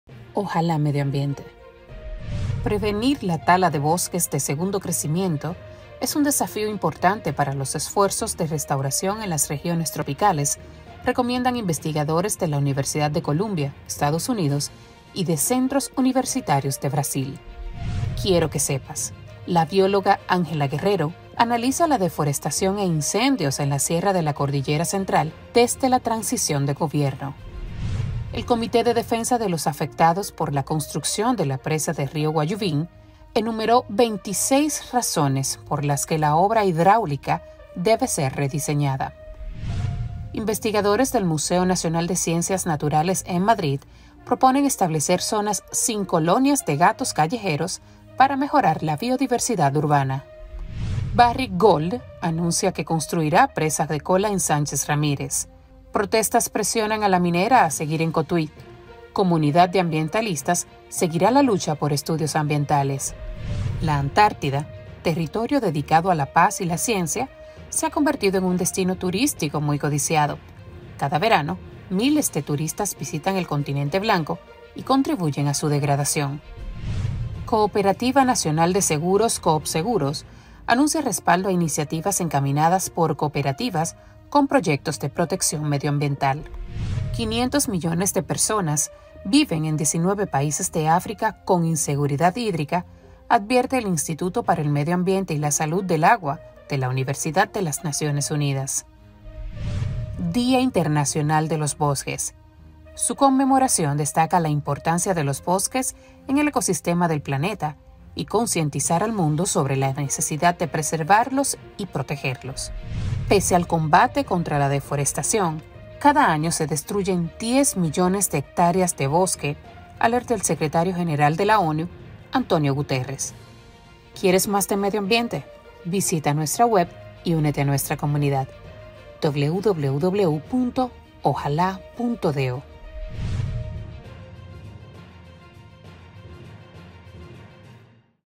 |Ojalá Medioambiente| Presa no va en Monte Plata
🔘Prevenir la tala de bosques de segundo crecimiento es un desafío importante para los esfuerzos de restauración en las regiones tropicales, recomiendan investigadores de la Universidad de Columbia (EEUU) y de centros universitarios de Brasil.
✔️Quiero que sepas: bióloga Ángela Guerrero analiza la deforestación e incendios en la Sierra de la cordillera central desde la transición de Gobierno.
🔘Comité de Defensa de los afectados por la construcción de la presa del río Guayubín, en Boca de Dos Ríos, enumeró 26 razones por las que la obra hidráulica debe ser rediseñada.
✔️Investigadores del Museo Nacional de Ciencias Naturales de Madrid proponen establecer zonas sin colonias de gatos callejeros para mejorar la biodiversidad urbana.
🔘Barrick Gold anuncia construirá presa de cola en Sánchez Ramírez. Protestas presionan a la minera a seguir en Cotuí. Comunidad de ambientalistas seguirá la lucha por estudios ambientales.
✔️La Antártida, territorio dedicado a la paz y la ciencia, se ha convertido en un destino turístico muy codiciado. Cada verano, miles de turistas visitan el Continente Blanco y contribuyen a su degradación.
🔘Cooperativa Nacional de Seguros (Coopseguros) anuncia respaldo a iniciativas encaminadas por cooperativas con proyectos de protección medioambiental.
✔️500 millones de personas viven en 19 países de África con inseguridad hídrica, advierte Instituto para el Medio Ambiente y la Salud del Agua de la Universidad de las Naciones Unidas.
🔘Día Internacional de los Bosques: su conmemoración destaca la importancia de los bosques en el ecosistema del planeta y concientizar al mundo sobre la necesidad de preservarlos y protegerlos.
✔️Pese al combate contra la deforestación, cada año se destruyen diez millones de hectáreas de bosque, alerta secretario general de la ONU, António Guterres.